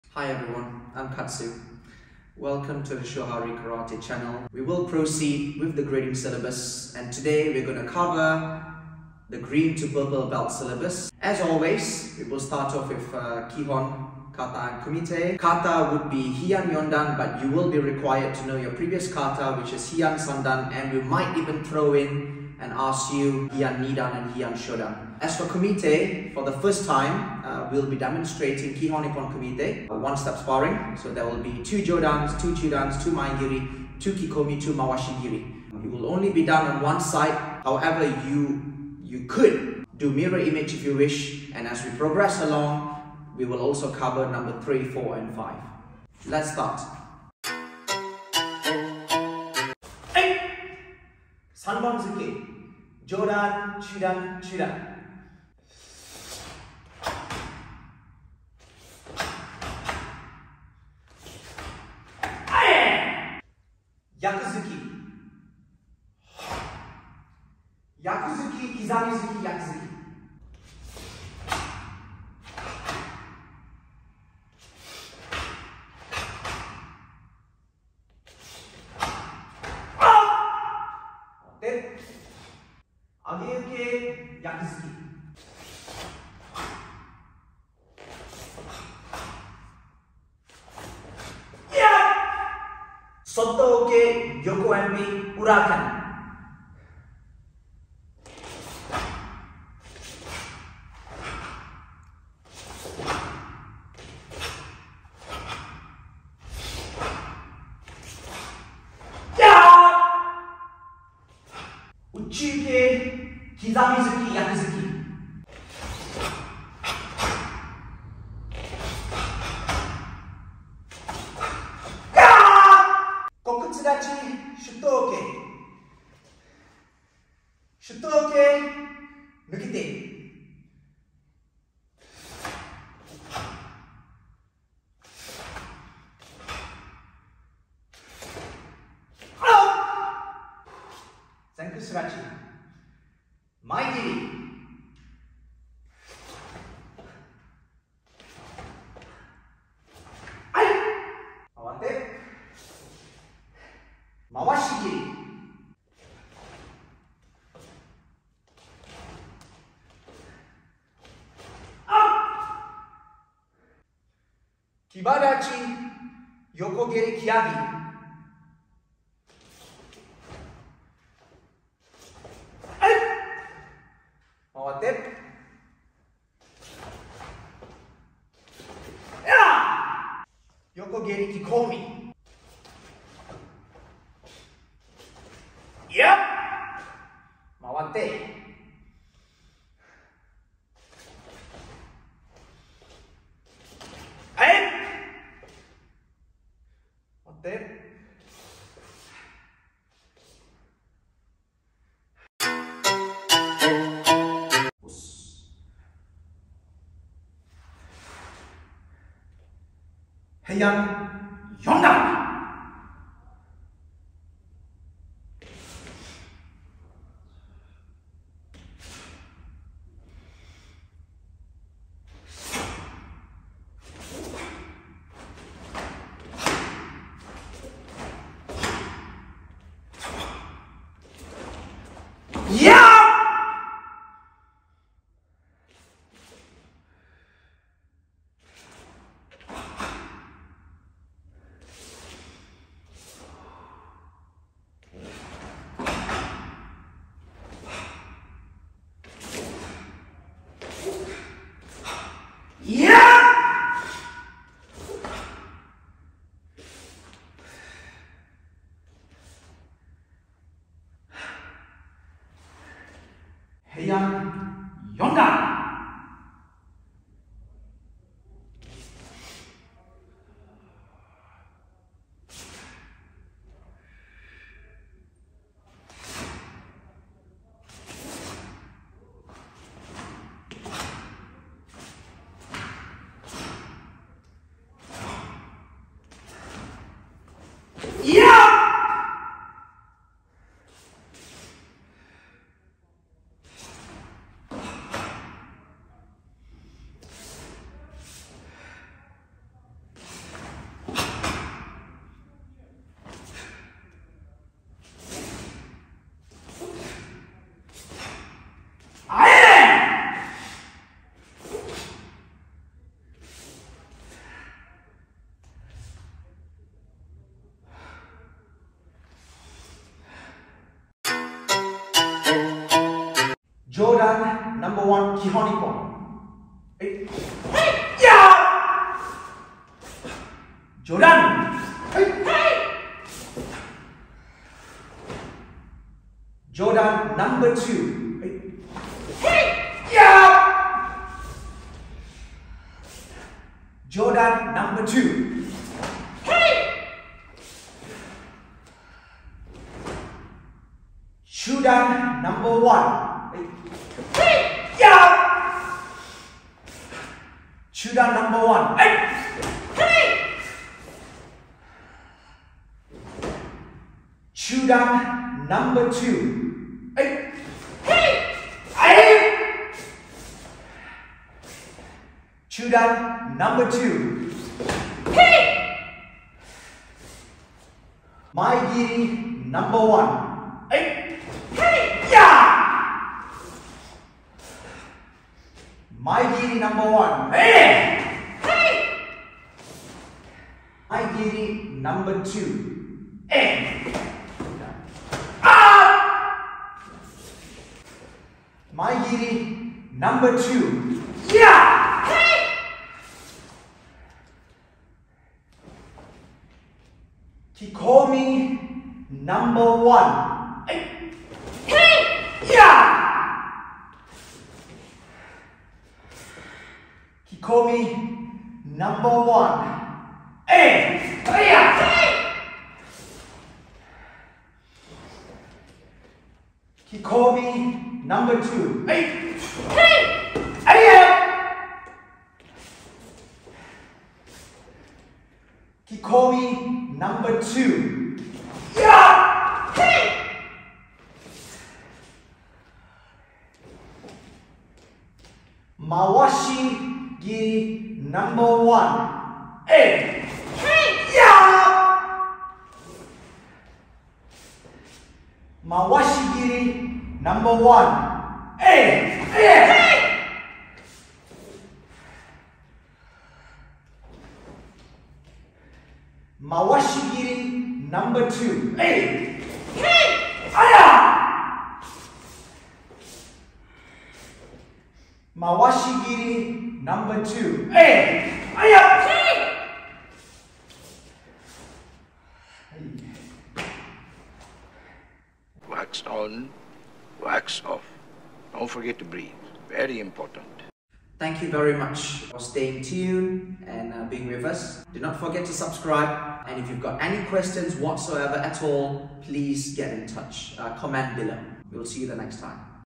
Karate grading syllabus 5th kyu (purple belt) in Shuhari Shotokan karate Association
This video contains basic karate technique which needs to know for 5th kyu Grading Syllabus (purple belt) in Shuhari Shotokan Karate Association.

Video also includes commentary from Katsu Sensei - Chief instructor of Shuhari Shotokan Karate Association